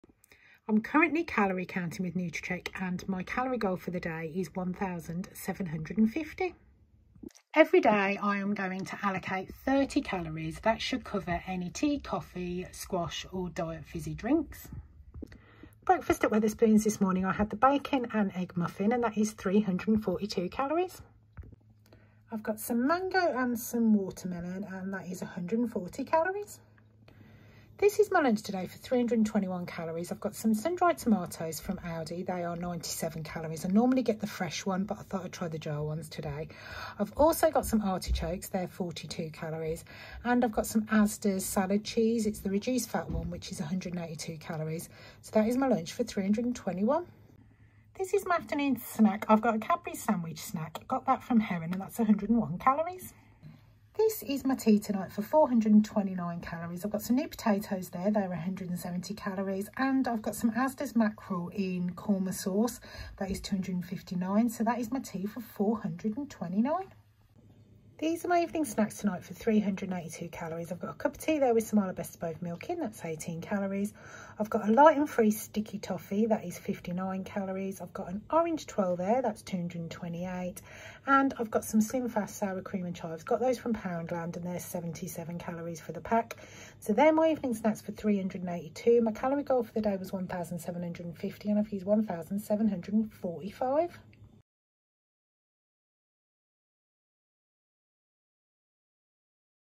250lbs lost. What I eat in a day calorie counting, using nutracheck. To lose weight. Using 1750 cals
Calorie counting using nutracheck. On 1750 cals a day

1745 cals used

Lost 260lbs
Started on slimming world and switched to ww uk. Now following calorie counting using nutracheck. I have been on my weight loss journey for over 6 years. Three years to lose the weight and been maintaining for over 3 years.